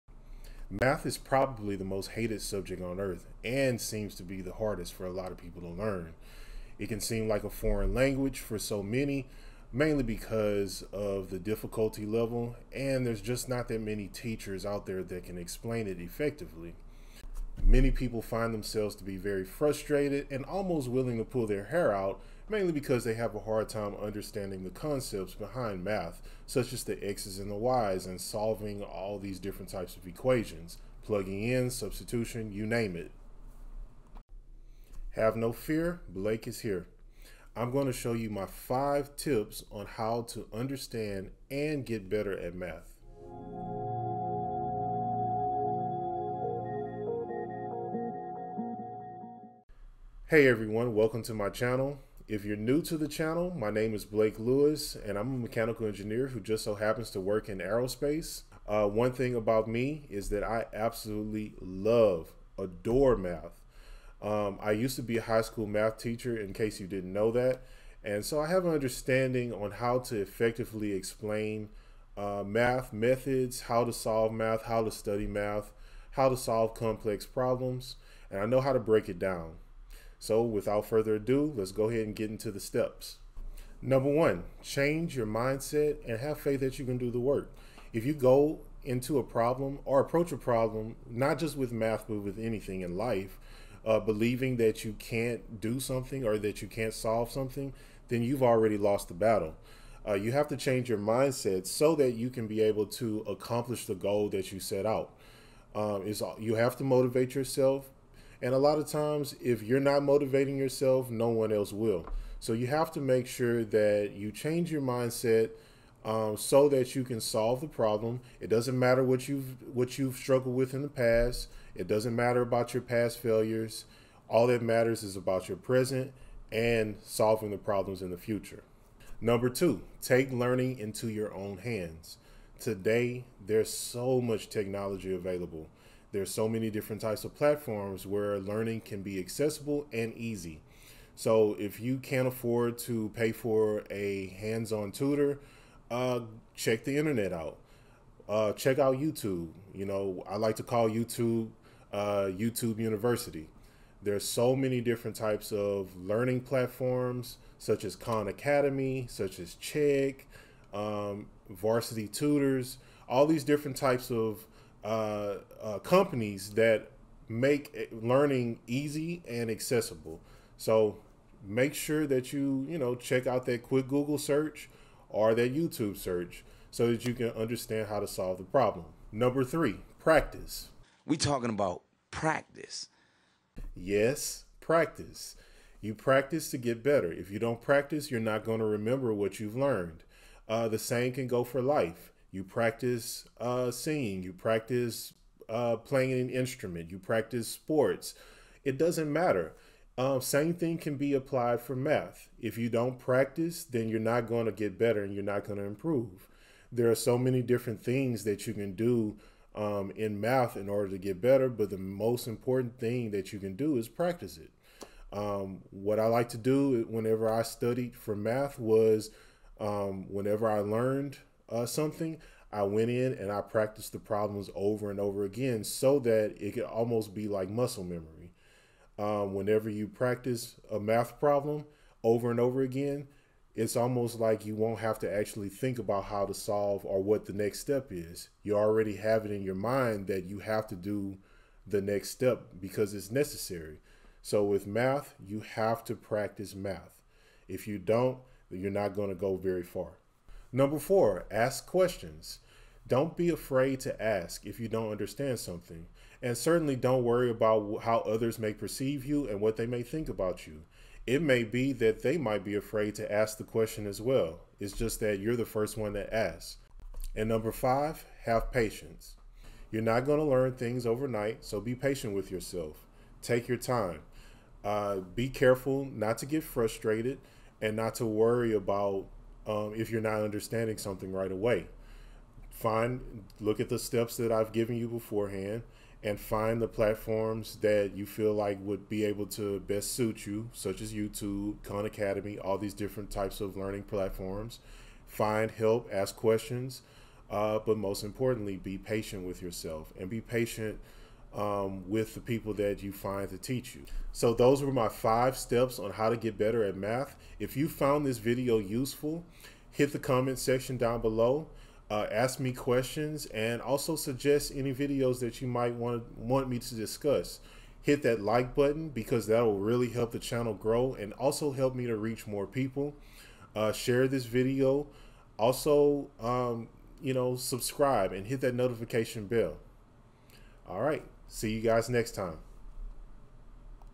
My 5 Steps to get better at Math
Math is one of the hardest subjects to learn for many students. I will explain my 5 steps and advice on how to get better at math.

Here are the steps:
1)  Change your mindset
2) Take learning into your own hands
3) Practice
4) Ask questions
5) Have patience